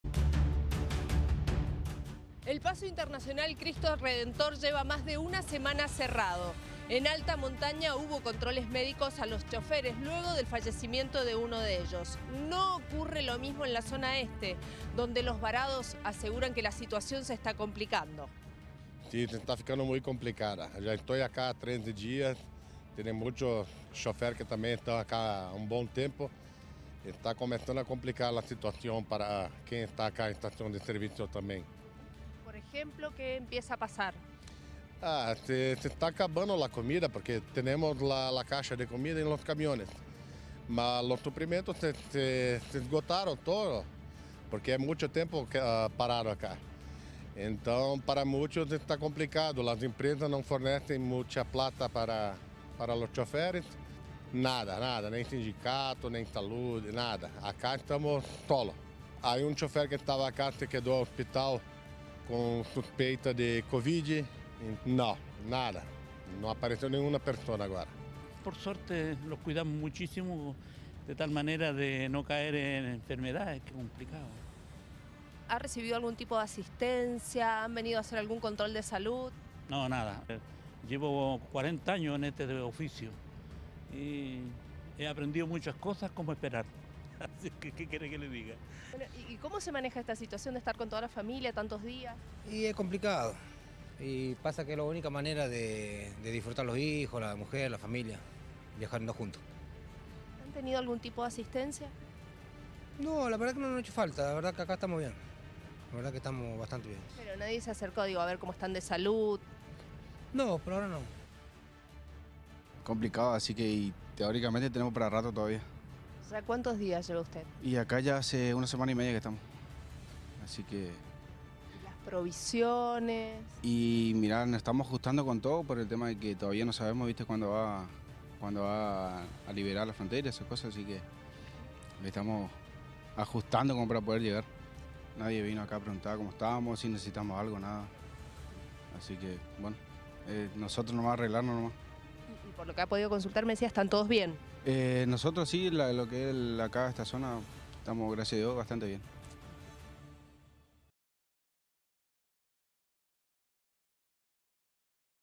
Camioneros se exponen a enfermedades debido a que están varados hace más de 3 días
Camioneros de distintos países se exponen a condiciones precarias al estar varados por más de 3 días debido al cierre del paso.